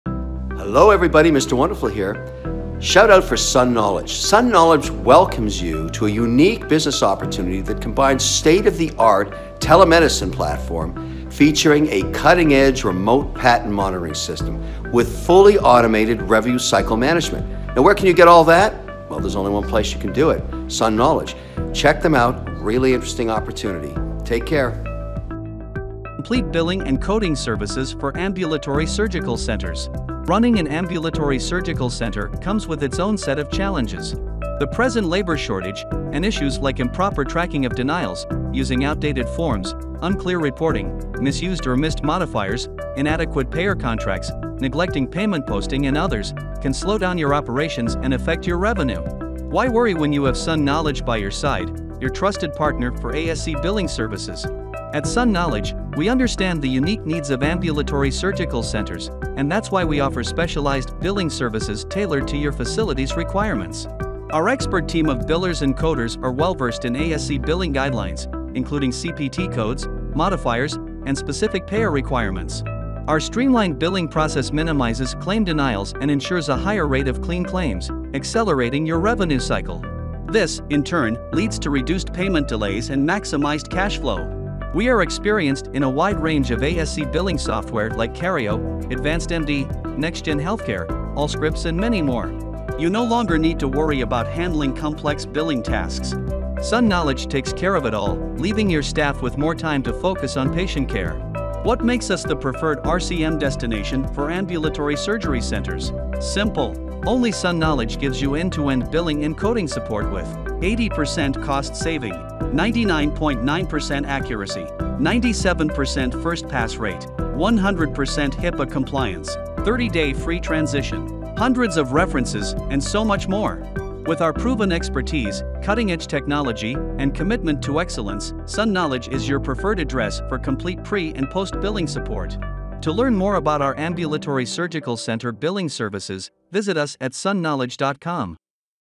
Ambulatory Surgical Center Billing Services Just $7/hr
Transforming the outpatient experience for millions of Americans, Ambulatory Surgery Center are a convenient alternative to hospital-based outpatient procedures and care. Ambulatory Surgery Center are completely out of traditional hospital settings, which eliminates the need for extended overnight hospital stays.
So in short, ambulatory Surgery Center popularly known as ASCs is a specialized medical facility focused on providing same day outpatient surgical care, including diagnostic and preventive procedures.